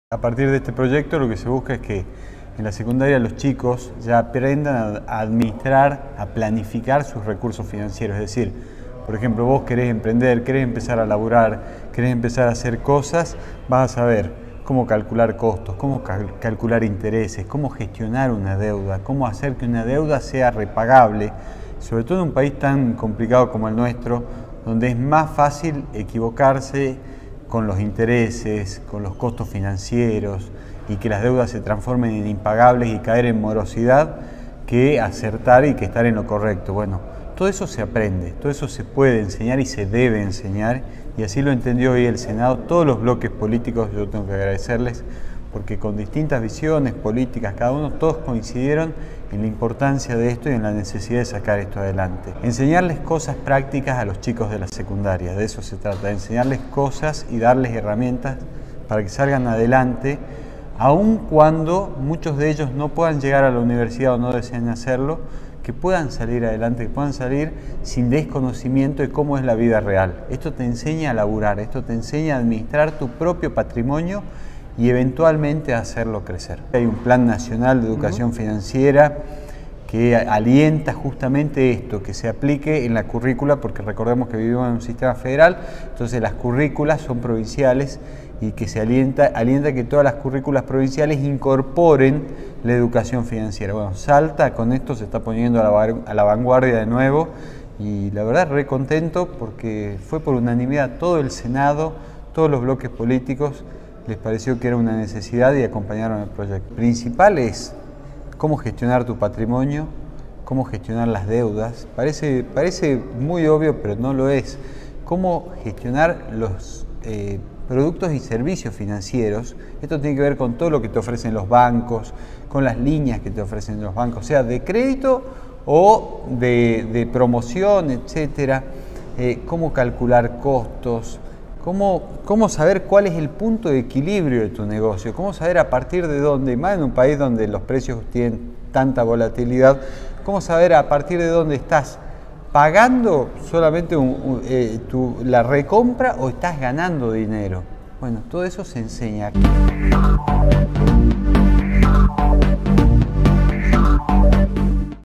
SALTA - El Senado aprobó la educación financiera para los estudiantes de escuelas secundarios#canal7
Emiliano Durand - senador por el Dpto. Capital